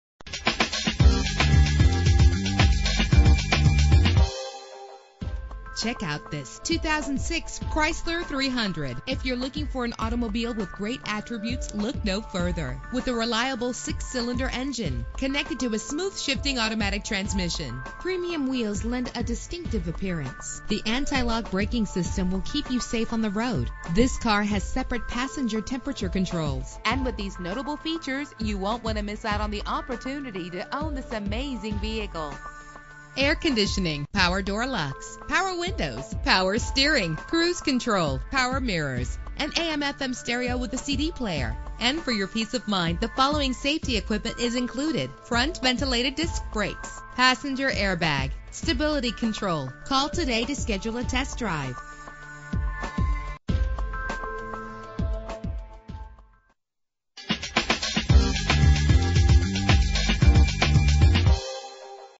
2006 Chrysler 300 - Budd Lake NJ
Year: 2006
Make: Chrysler
Model: 300
Trim: Touring
Engine: 3.5 liter 6 cylinder 24 valve
Transmission: 4-Speed Automatic
Color: Gray
Mileage: 88719
Address: